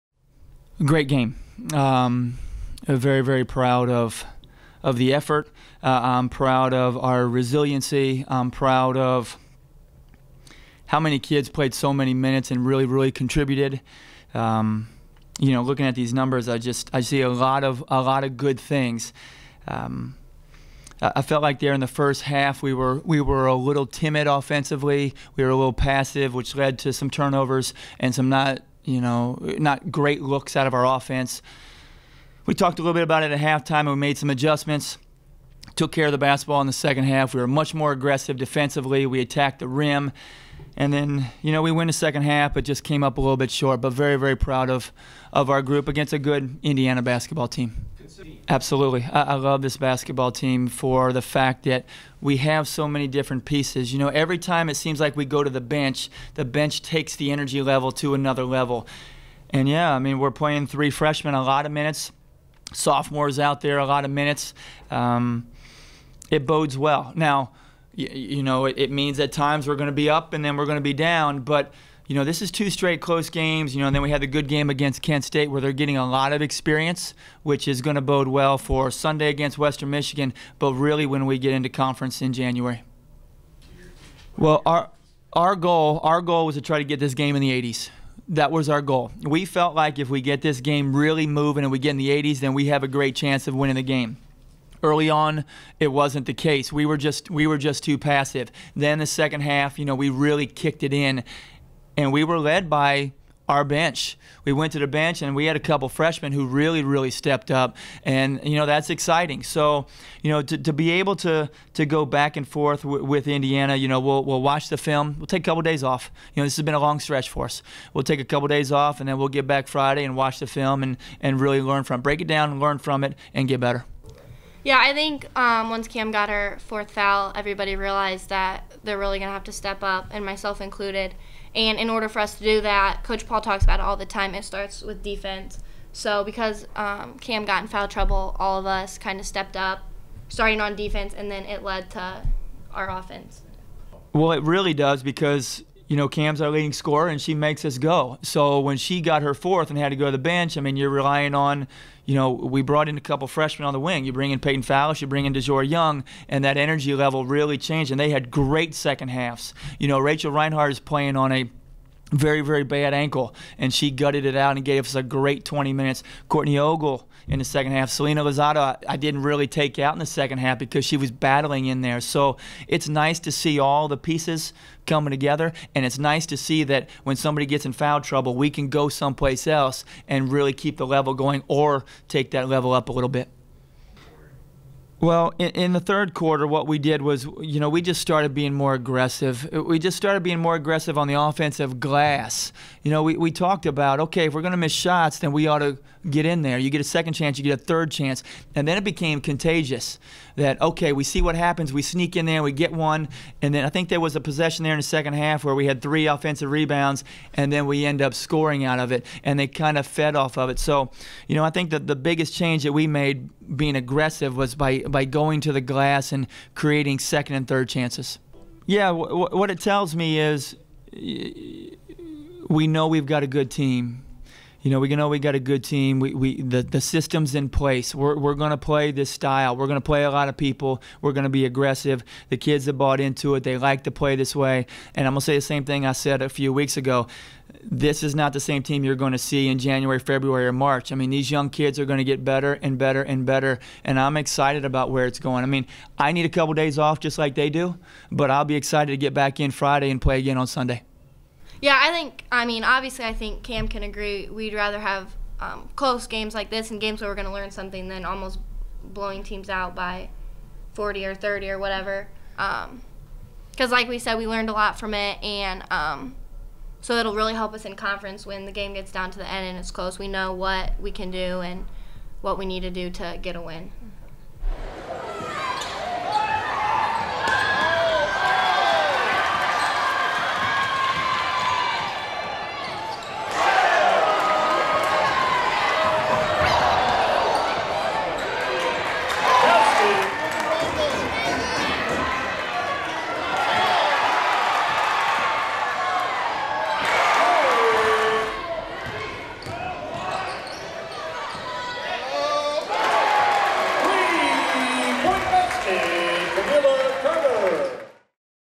IPFW Mastodons Fall to IU in Last Minute Thriller
IPFW Women's Basketball vs Indiana University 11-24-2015

FORT WAYNE, Ind. - The IPFW women's basketball team fought a tough battle against Indiana, but the Mastodons came up short, falling 79-72 at home to the Hoosiers.

Kamilah Carter scored a team-high 23 points, with 17 coming in the first half. Carter also pulled down five rebounds and made a game-high three steals. De'Jour Young was the only other Mastodon player in double-digit scoring with 13.

Rachel Rinehart had a team-high four assists and five rebounds, while KeShyra McCarver added eight points and five rebounds for the 'Dons.

IPFW was in the game from the start, winning the opening tip, and scoring the first points of the night with a layup from Carter. Josie Fisher played well defensively in the first half, forcing turnovers and helping the Mastodons to score seven points off turnovers in the first quarter.

The 'Dons were able to cut the Hoosier's lead to seven at the 2:00 mark of the second quarter, but went into the half trailing 45-32.

After a fast-paced first half, IPFW outscored Indiana in the third quarter, 21-15, to cut the Hoosier's lead down to 60-53 at the end of the period.

The fourth quarter saw the 'Dons get into foul trouble, as three players eventually foul out of the game in Carter, Fisher, and Rinehart. The Mastodons pressured the Hoosiers in the final minutes, going on an 11-2 run, but were only able to pull within four with 47 seconds remaining.

The Mastodons connected on 27-of-72 (37.5%) from the field, including 5-of-19 (26.3%) from long range, and 13-of-18 (72.2%) from the charity stripe.

IPFW is back at home on November 29 for a 2 p.m. tip-off against Western Michigan.
~ Feel the Rumble ~